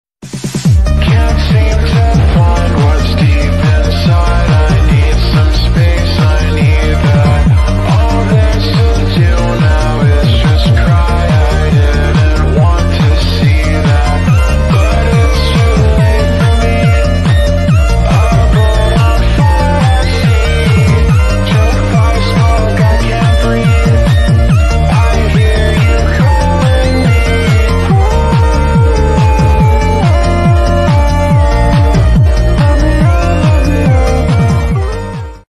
☆ .space. ☆ ( half vent / stress relief ) || Backrooms Partypooper💙 OC
had a burnout for a day sorry for not posting =(
im also not proud of this the animation is really janky and weird..

apps: flipaclip, capcut
fps: 16
time taken: 7 hours

character: partypooper / entity 68 (backrooms)
oc: sorrow

yeah i changed his design cause his old one looked boring lol